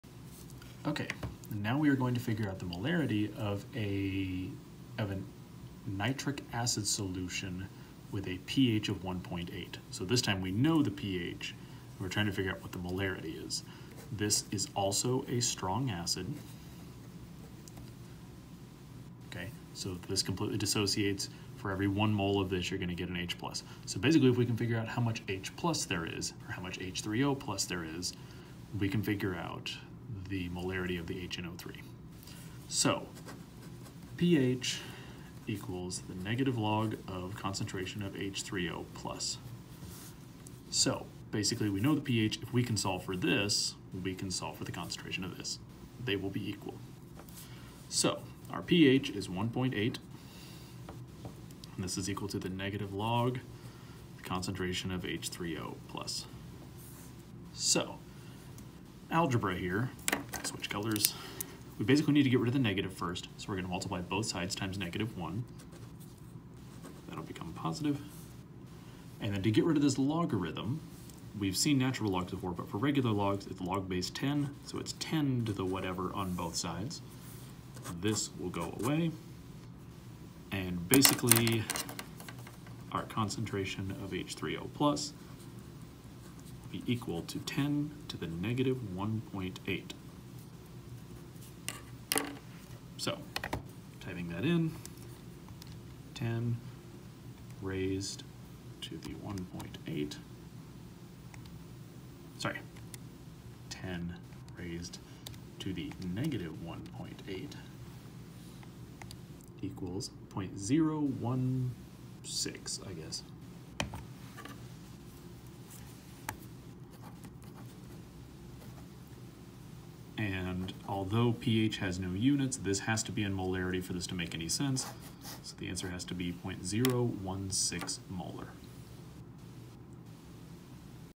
Molarity of strong acid from pH
Molarity of HNO3 of solution with pH of 1.8.